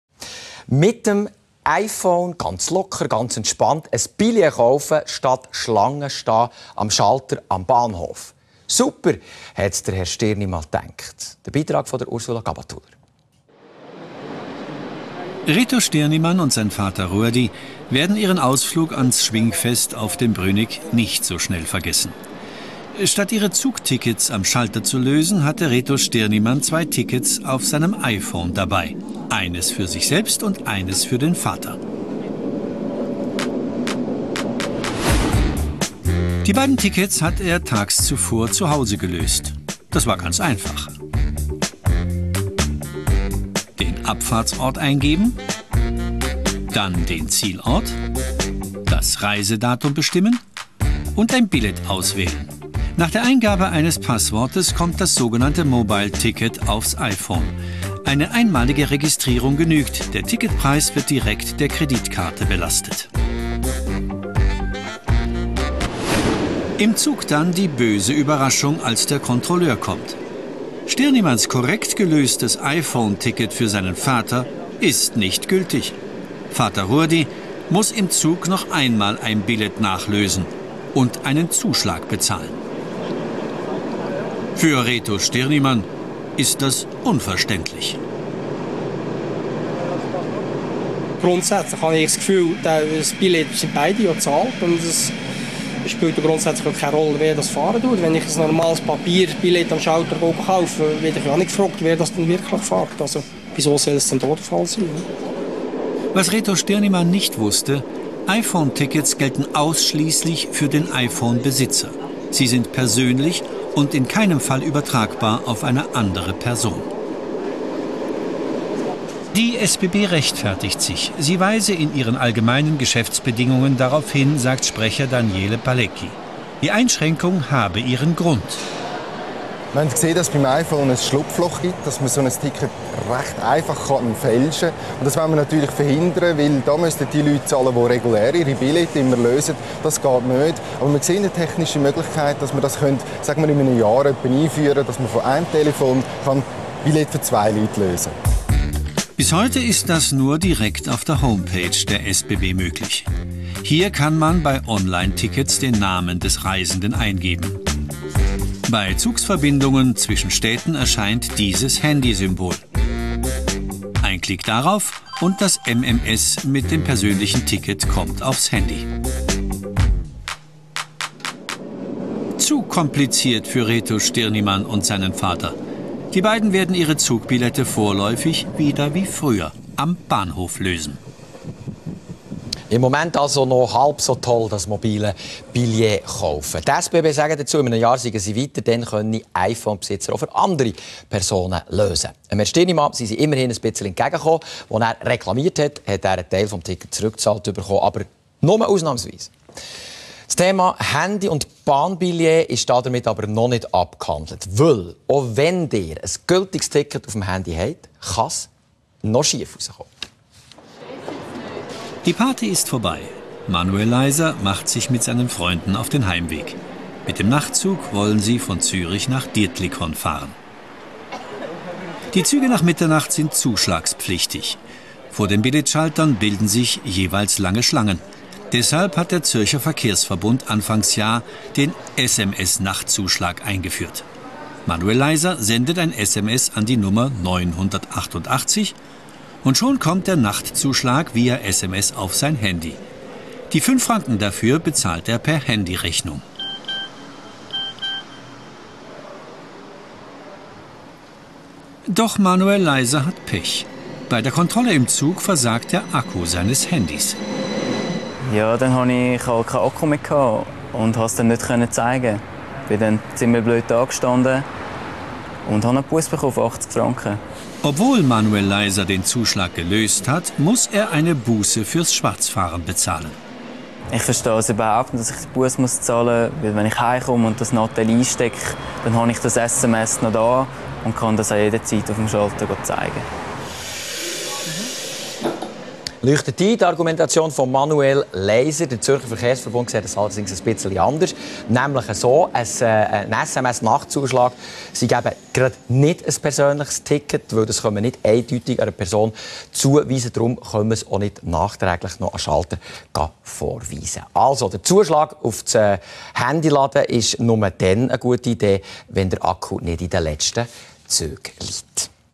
iPhone SBB Tickets sind nicht übertragbar
SBB Tickets die mit der iPhone App der SBB gekauft wurden sind nicht übertragbar!